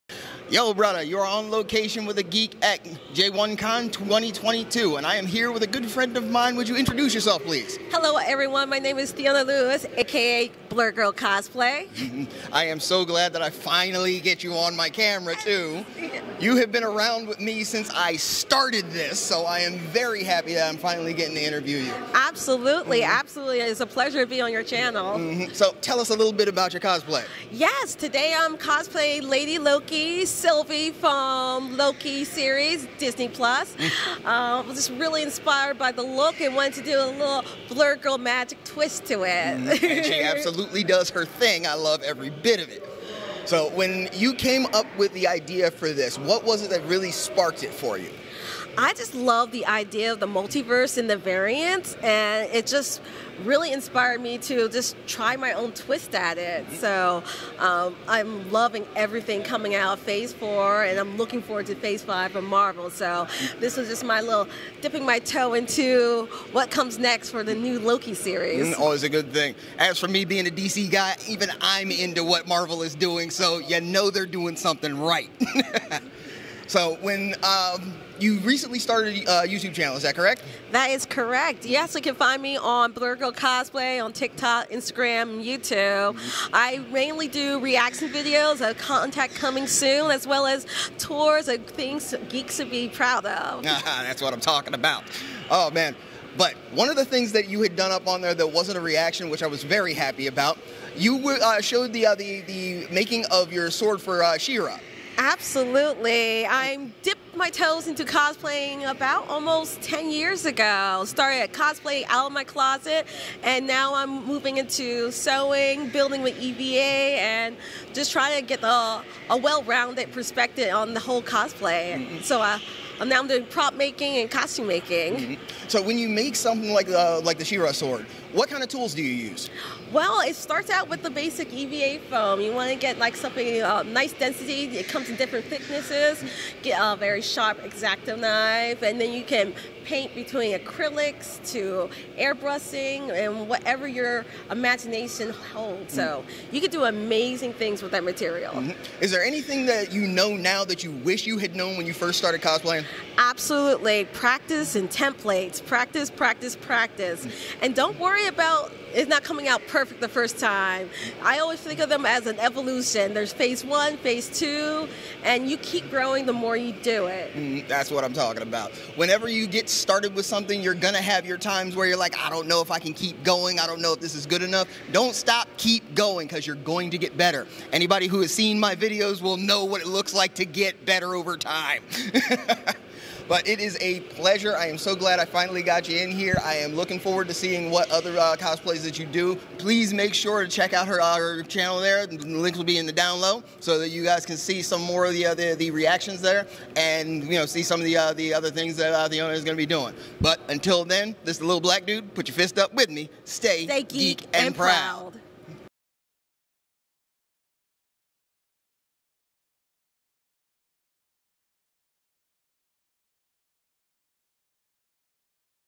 J1-Con 2022 Cosplayer Interview with Theona Lewis
An interview years in the making! One of the original Sistahs of the channel, Theona Lewis AKA Blerd Girl Cosplay is on camera! She is a YouTuber, TikTokker and cosplayer and is showing off her Sylvie cosplay. She talks inspiration, creativity and what makes a good cosplay.